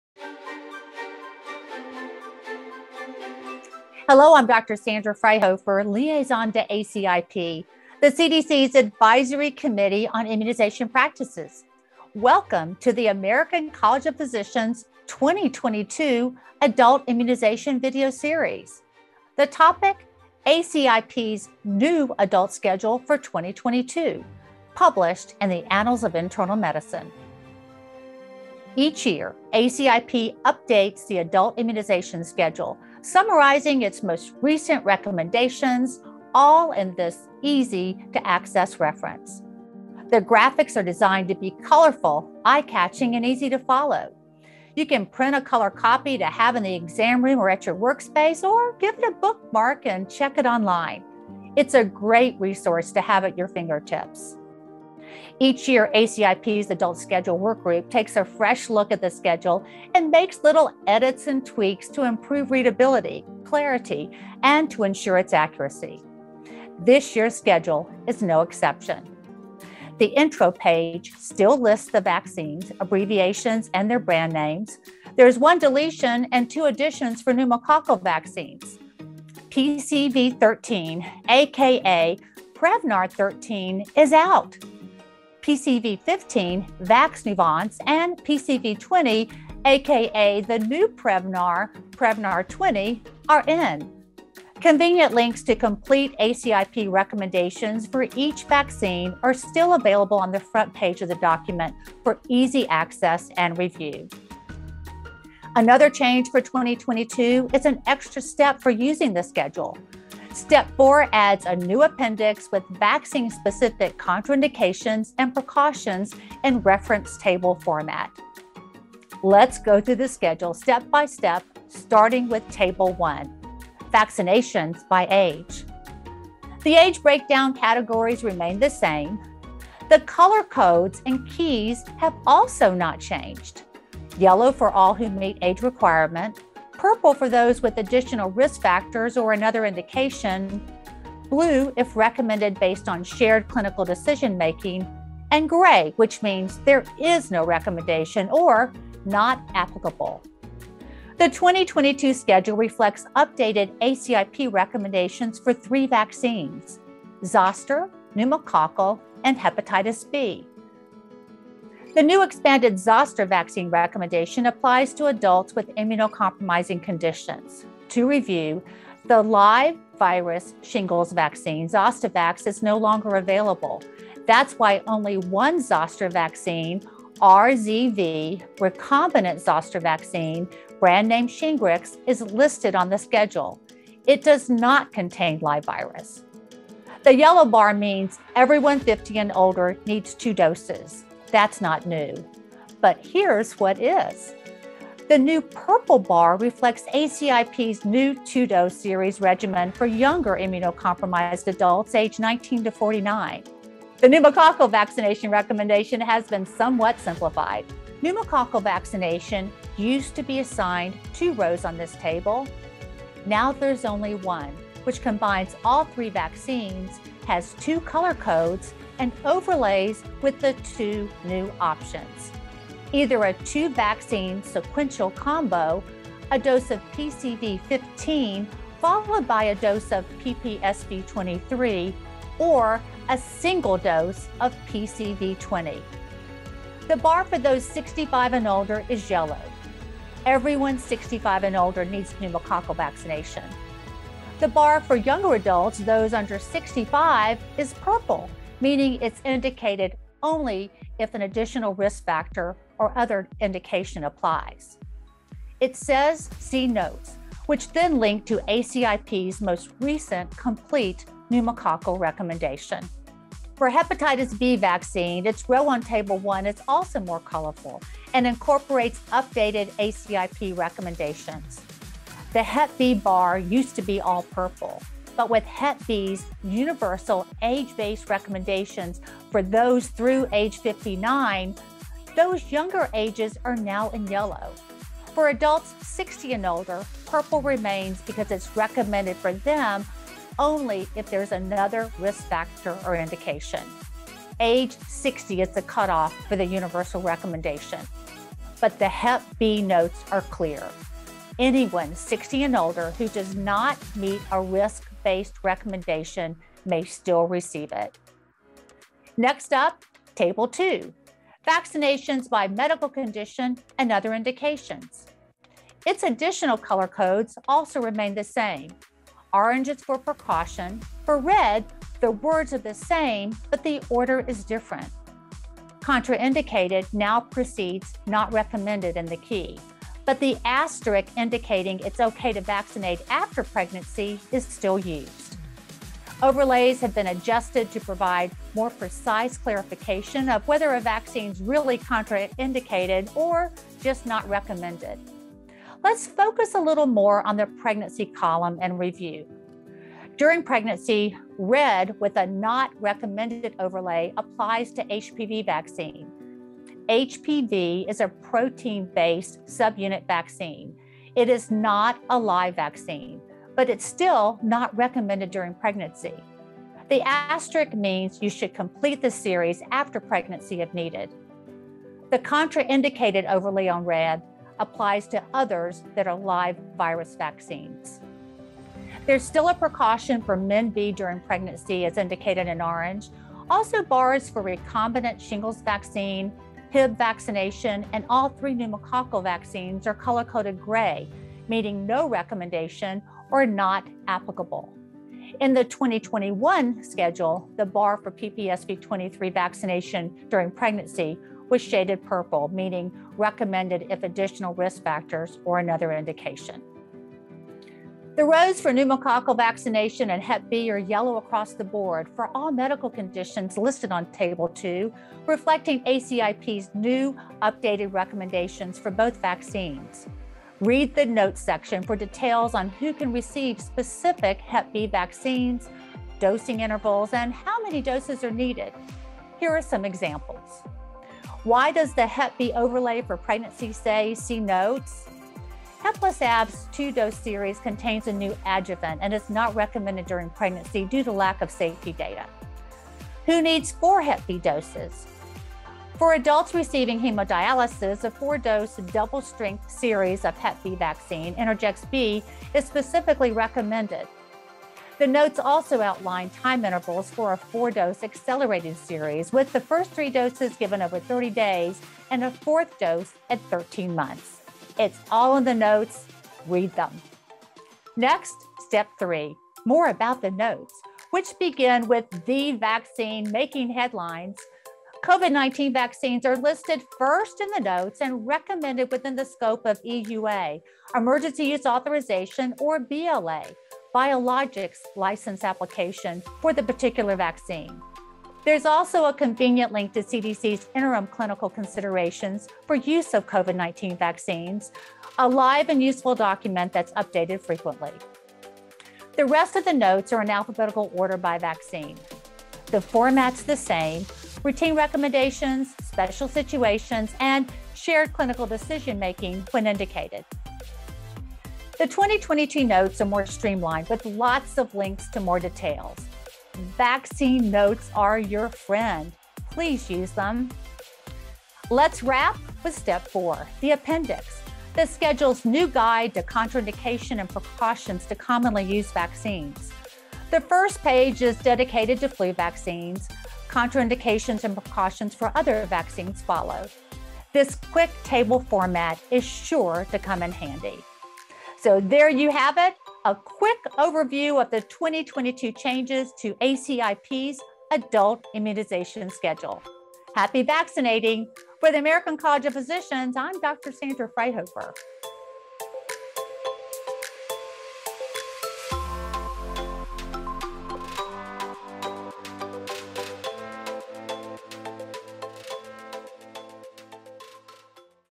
ACIP Adult Immunization Schedule 2022 | American College of Physicians
In this video, Dr. Sandra Adamson Fryhofer, MD, MACP explains the 2022 Advisory Committee on Immunization Practices’ Adult Immunization Recommendations.

These videos were developed through ACP's I Raise the Rates program supported by the Centers for Disease Control and Prevention of the U.S. Department of Health and Human Services and Sanofi Pasteur.